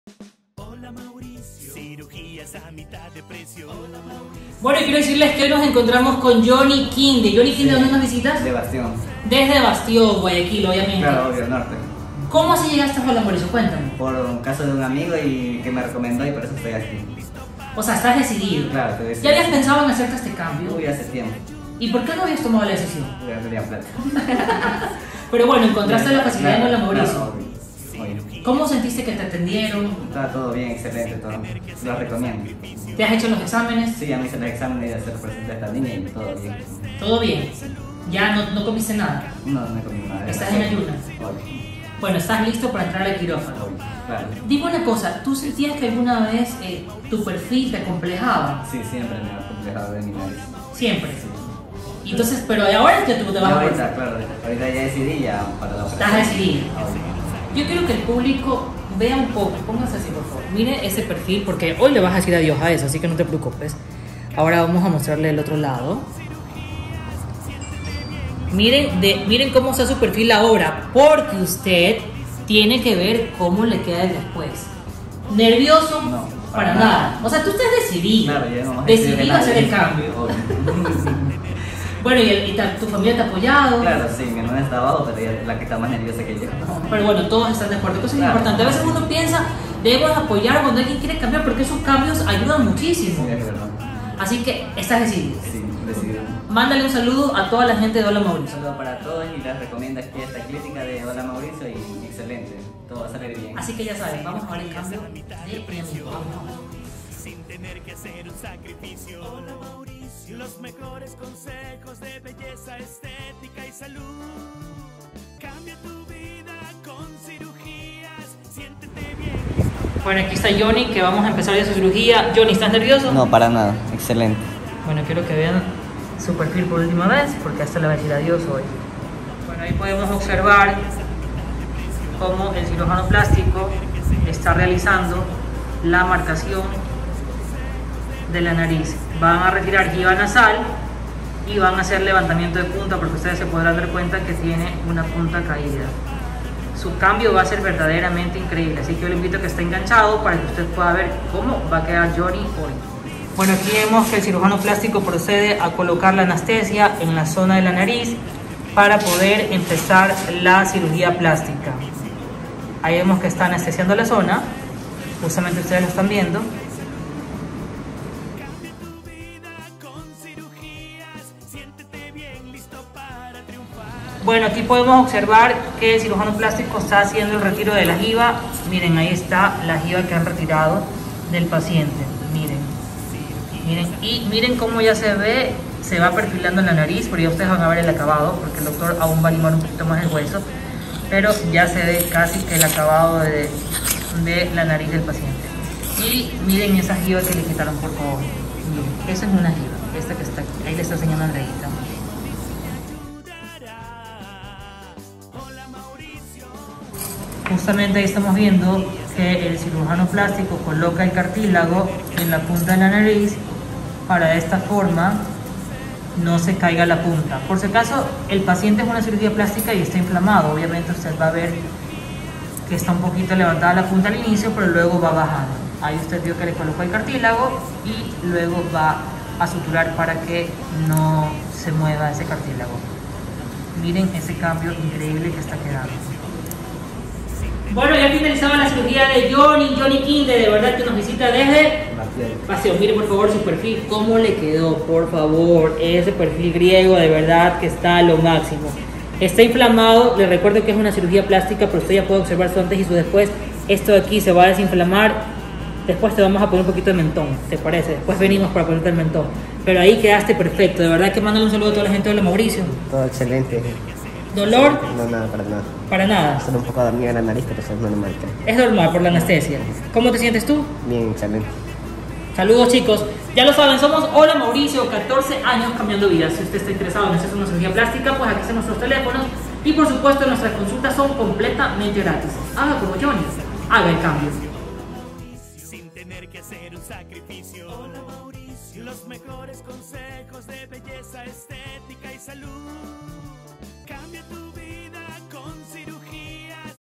Jhony Quinde y su cambio
Este es el casi de jhony un chico de bastión popular que reunió su dinero para poder hacer su sueño cambiar su nariz mira su increíble cambio claro con holamauricio ecuador ☎️☎️☎️ #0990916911 consultas gratis